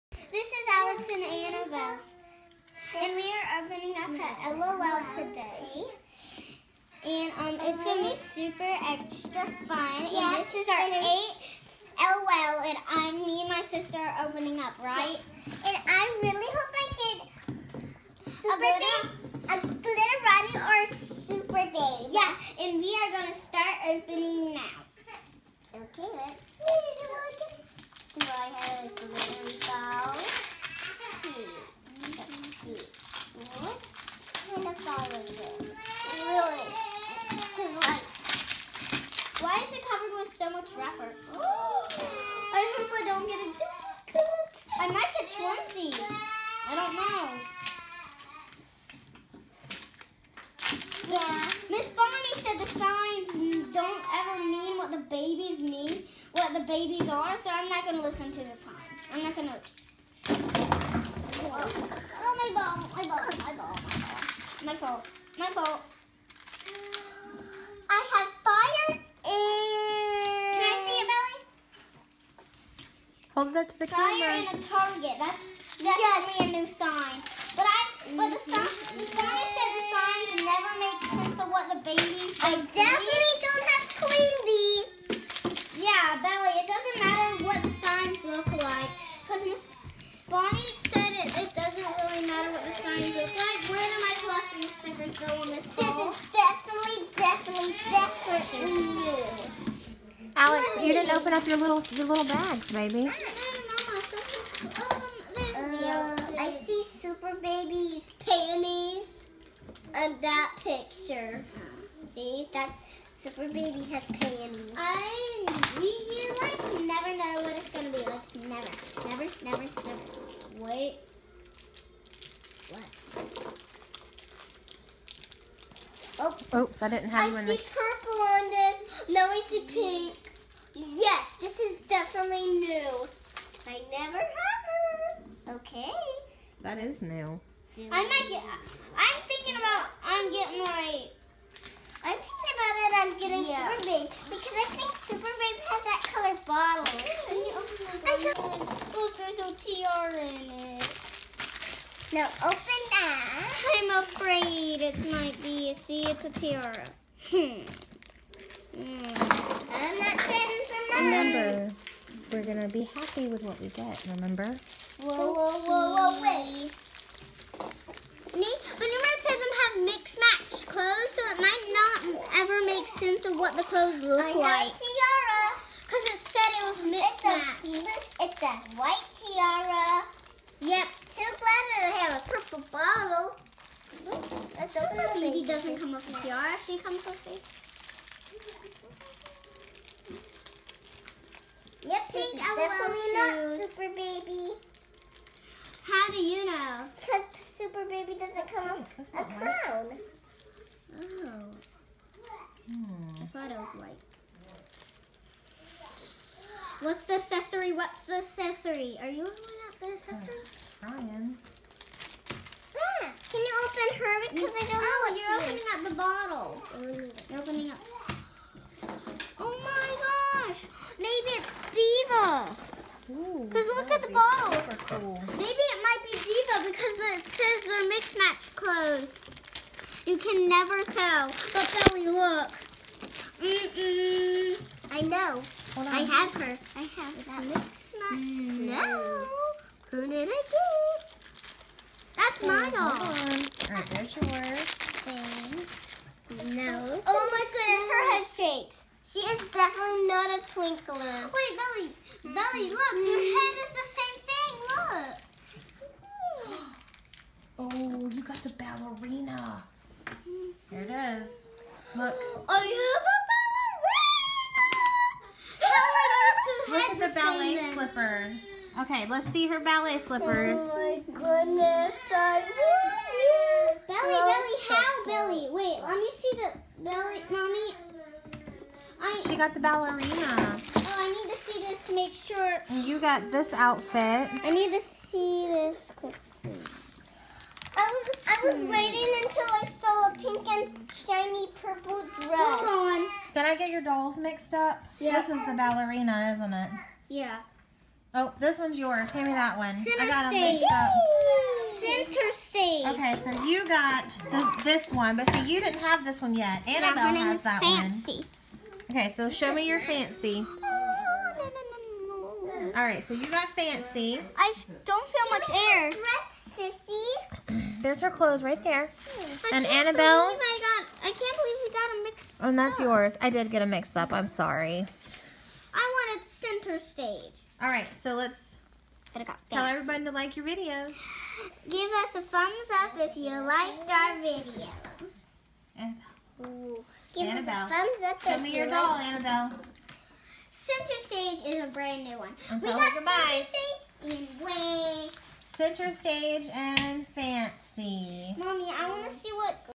LOL SURPRISE FANCY AND CENTER STAGE
Watch as Alex and Annabelle open new lol dolls